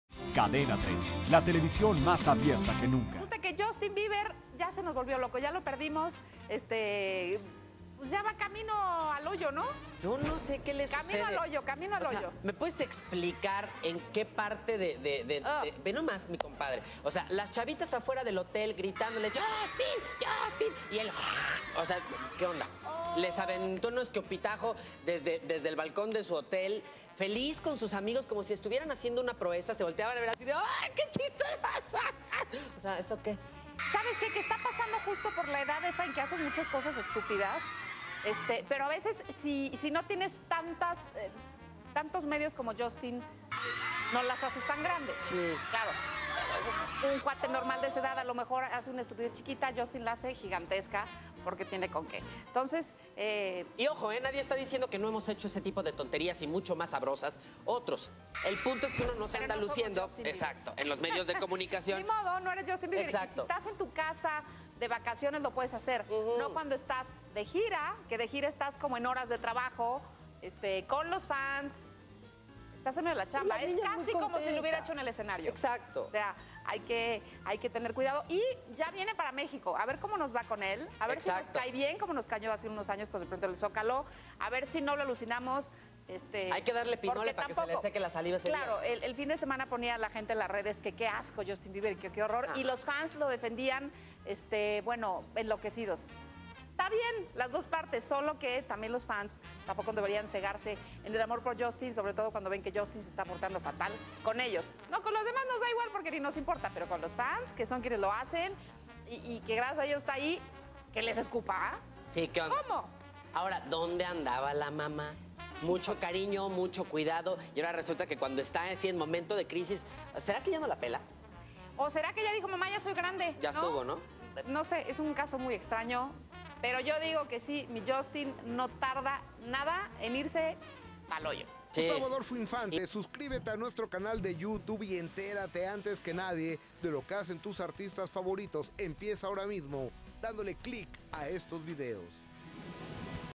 Justin Bieber escupe a sus fans / Justin Bieber spits his fans
El famoso cantante Justin Bieber escupió a sus fans desde un balcón. El cantante se encontraba acompañado por un grupo de amigos. ¡No te lo puedes perder!